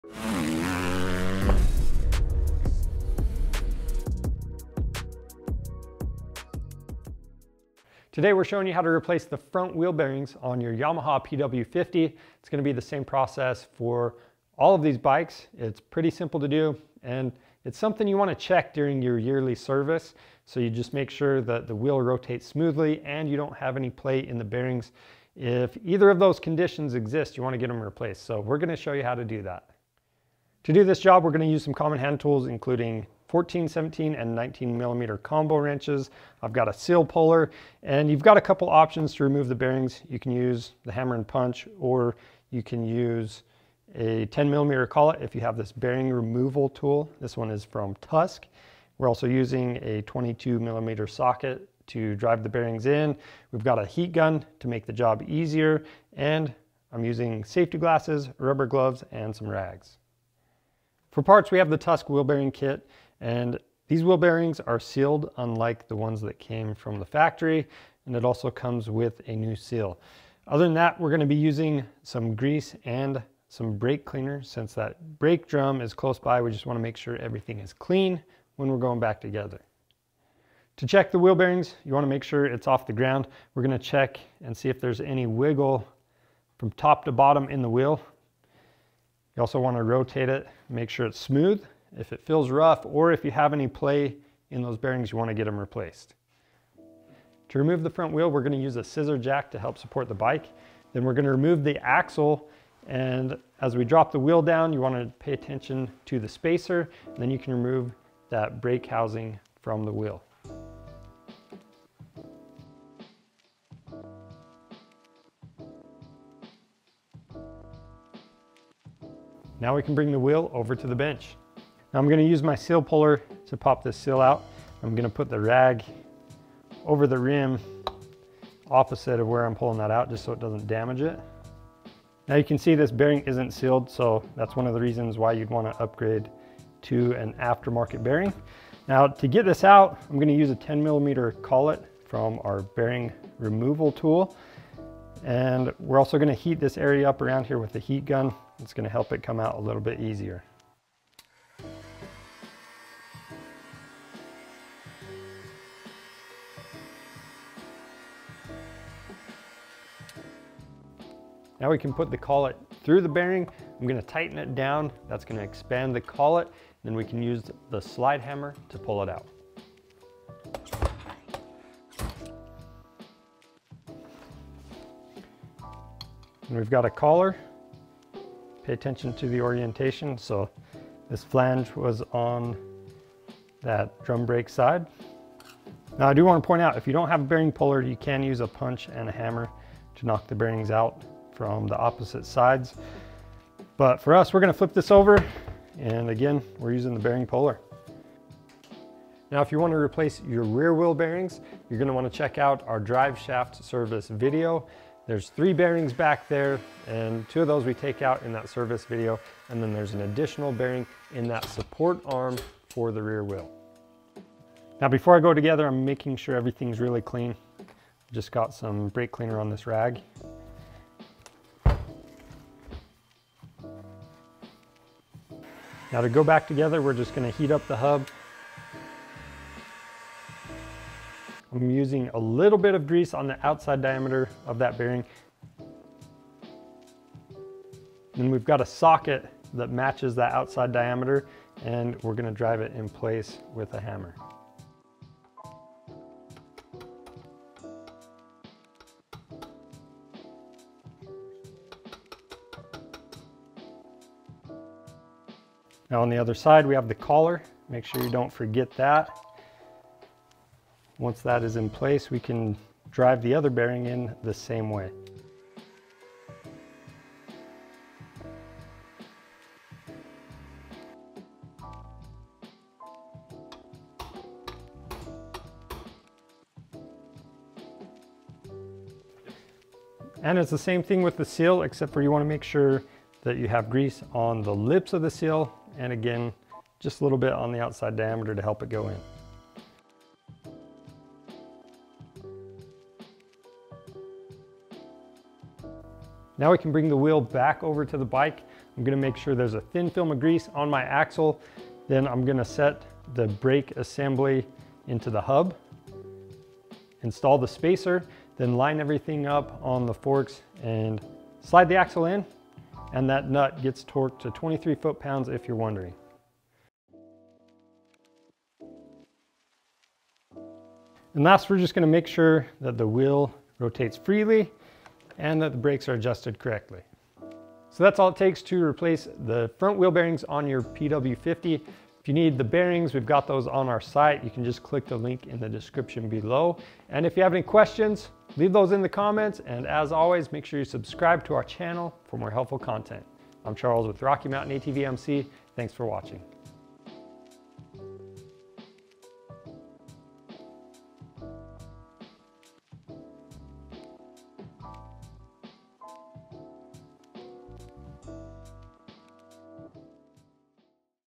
How To Replace Front Wheel Bearings on a Yamaha PW50
Inspecting and replacing the wheel bearings on your PW50 is a part of regular maintenance that will need to be done over the course of owning your bike. Knowing how to do the job the right way will help save you time and money over taking your bike to a shop. So, in this video learn how to inspect and replace the front wheel bearings on your PW as Charles shows the proper procedures on a Yamaha PW50.